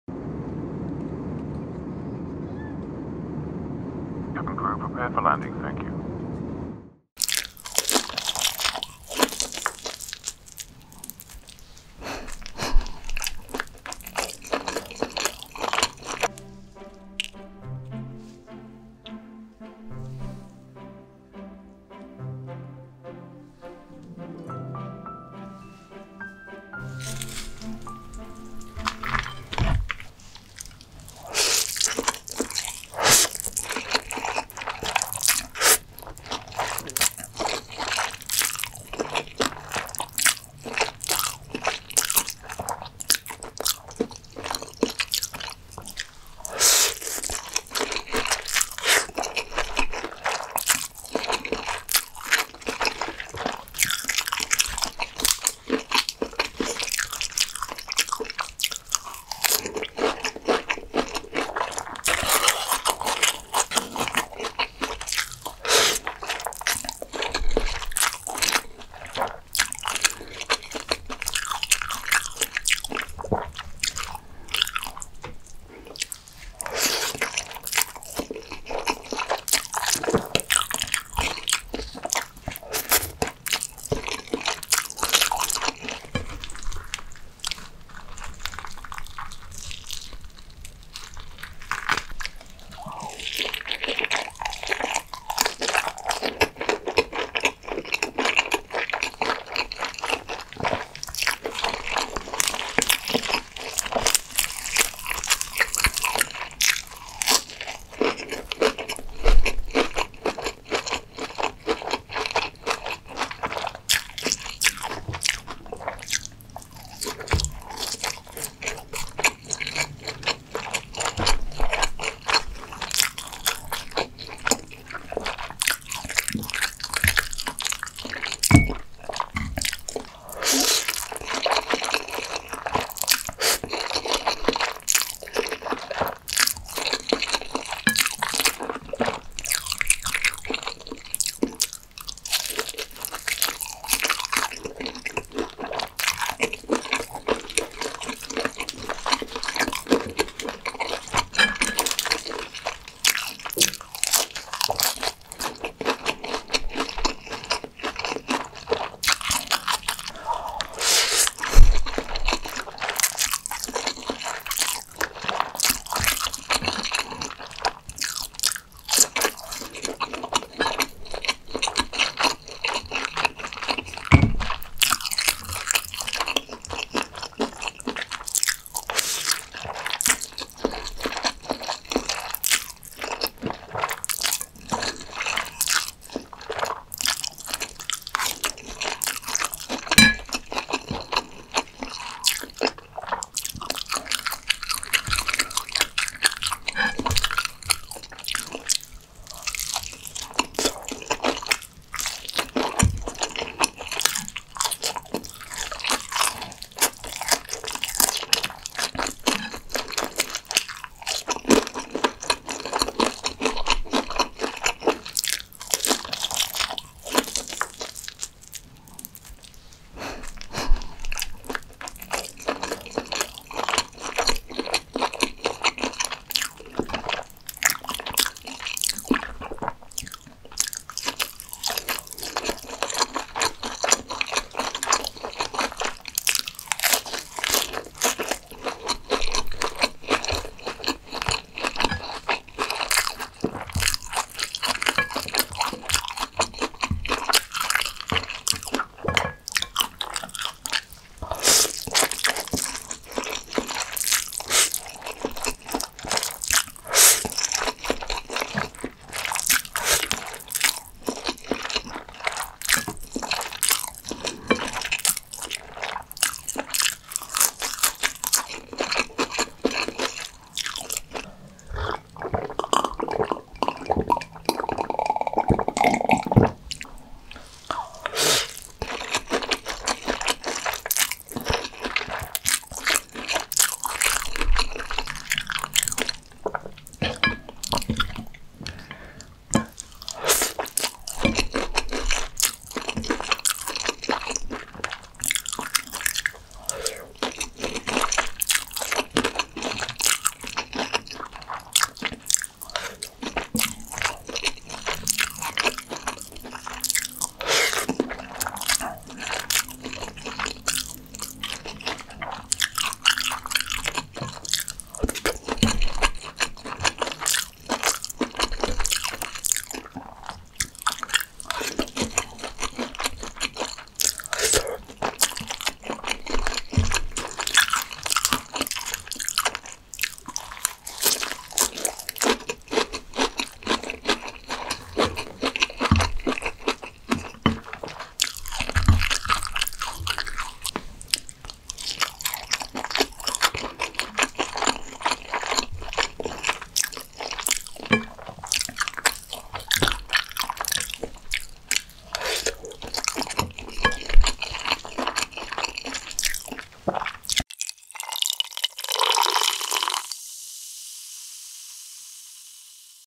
ASMR 🍜🍗MIE SEDAAP TASTY BAKMI AYAM & AYAM GORENG | CHICKEN NOODLE & FRIED CHICKEN | EATING SOUND
Your subscription would really mean a lot to me 🥰❤

CREDITS:
Sound opening-ending by zapsplat